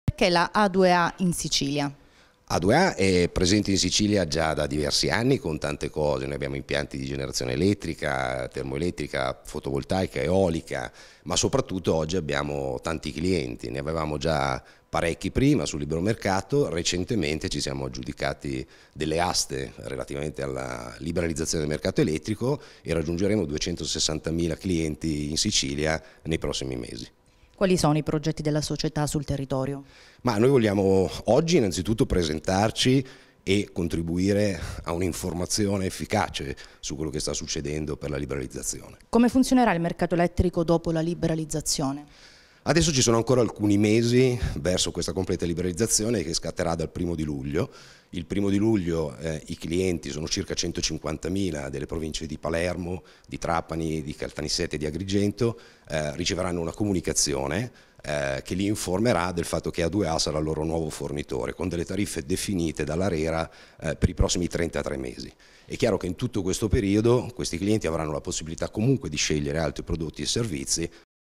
A2A: Verso il libero mercato dell’energia elettrica
Una corretta informazione sulle opportunità della concorrenza nel mercato dell’energia elettrica e accompagna i cittadini nel passaggio al mercato libero. Incontro a Palermo organizzato da A2A.